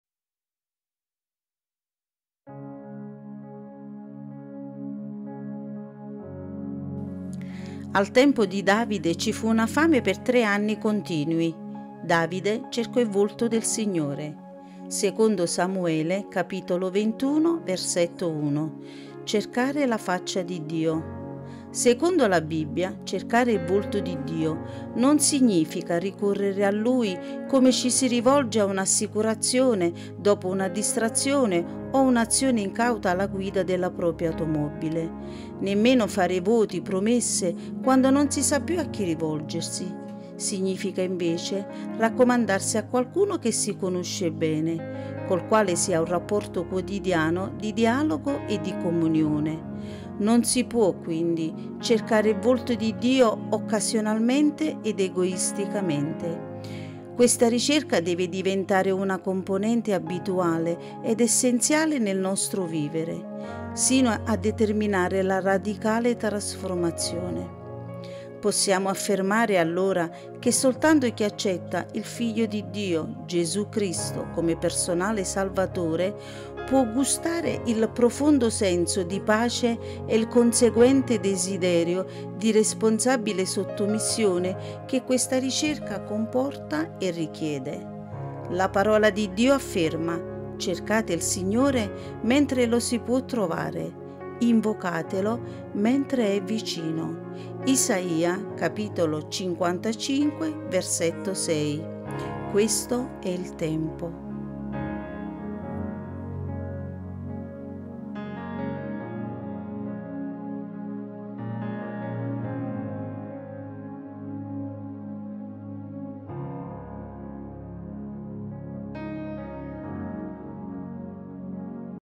"Cercare La Faccia di Dio" - Meditazione 2 Samuele 21:1
"Al tempo di Davide ci fu una fame per tre anni continui; Davide cercò il volto del Signore..." (2° Samuele 21:1)

CERCARE LA FACCIA DI DIO

Secondo la Bibbia, cercare il volto di Dio non significa ricorrere a Lui come ci si rivolge ad una assicurazione dopo una distrazione o un'azione incauta alla guida della propria automobile, né fare voti e promesse quando non si sa più a chi rivolgersi; significa invece raccomandarsi a qualcuno che si conosce bene, col quale si ha un rapporto quotidiano di dialogo e di comunione. Non si può, quindi, cercare il volto di Dio occasionalmente ed egoisticamente; questa ricerca deve diventare una componente abituale ed essenziale del nostro vivere, sino a determinare la radicale trasformazione. Possiamo affermare, allora, che soltanto chi accetta il Figliolo di Dio, Gesù Cristo, come personale Salvatore può gustare il profondo senso di pace e il conseguente desiderio di responsabile sottomissione che questa ricerca comporta e richiede. La Parola di Dio afferma: "Cercate il Signore, mentre lo si può trovare; invocatelo, mentre è vicino" (Isaia 55:6). Questo è il tempo!